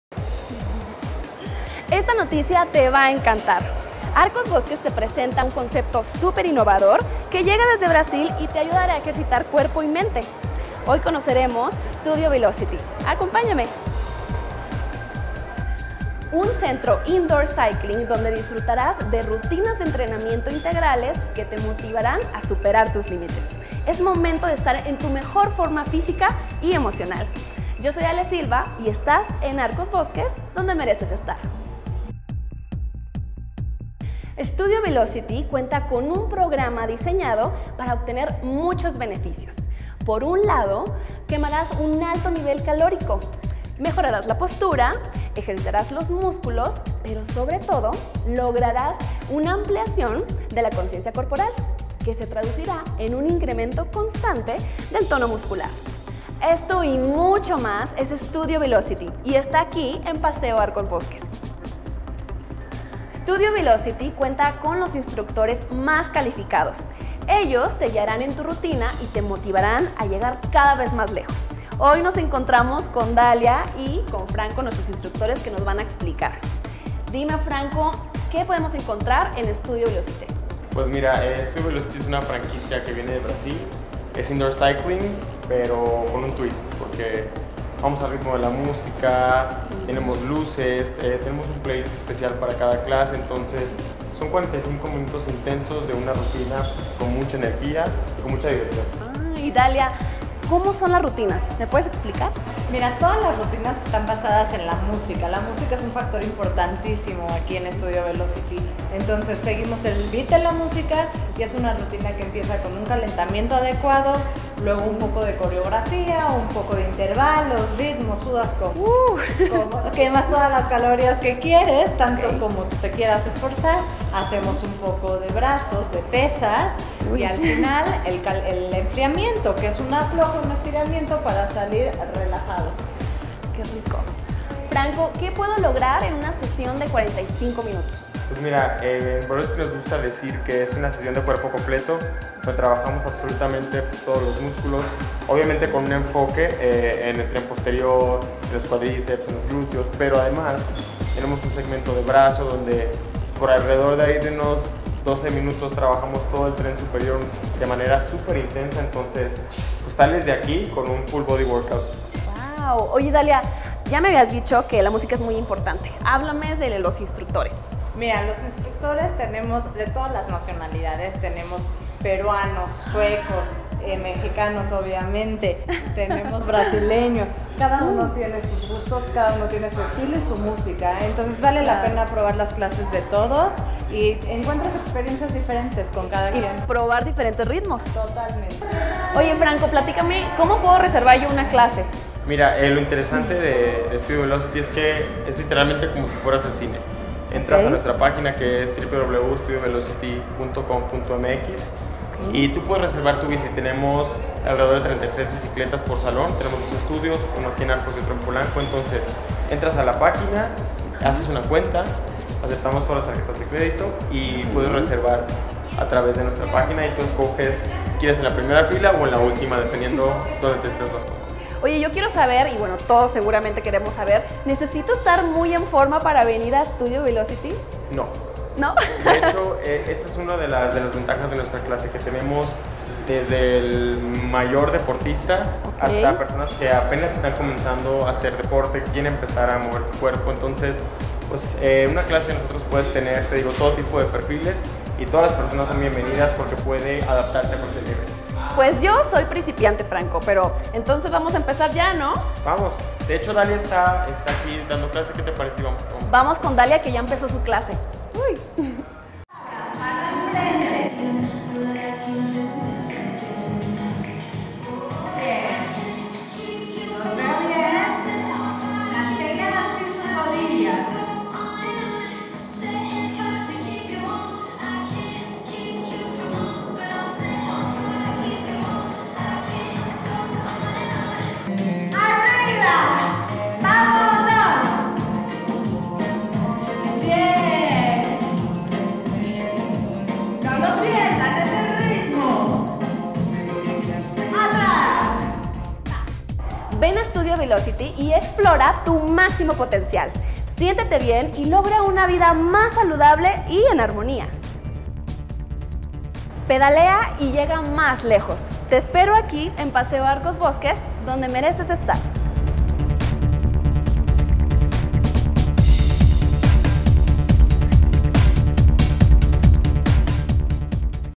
Arcos Life / Indoor Cycling Directo De Brasil / Studio Velocity
No puedes dejar de tener esta experiencia, modela tu cuerpo con este concepto brasileño de indoor cycling y esta en exclusiva en Paseo Arcos Bosques..Studio Velocity